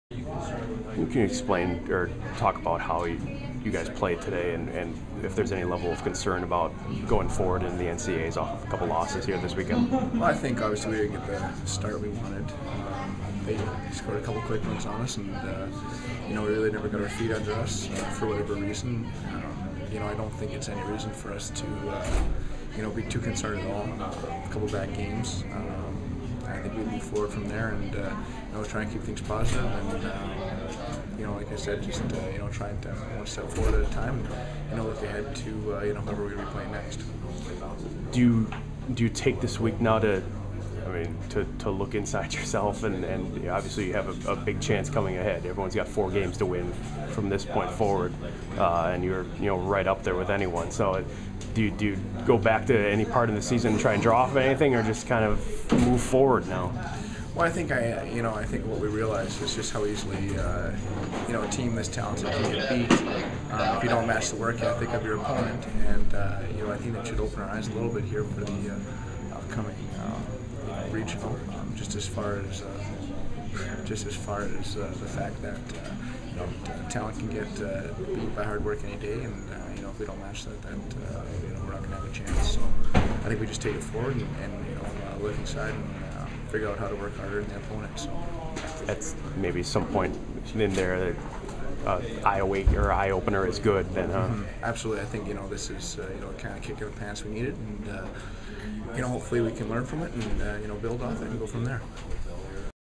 Denver's Jesse Martin, March 20, 2010
Denver's Jesse Martin talks about his team's 6-3 loss to Wisconsin in the WCHA Final Five third-place game.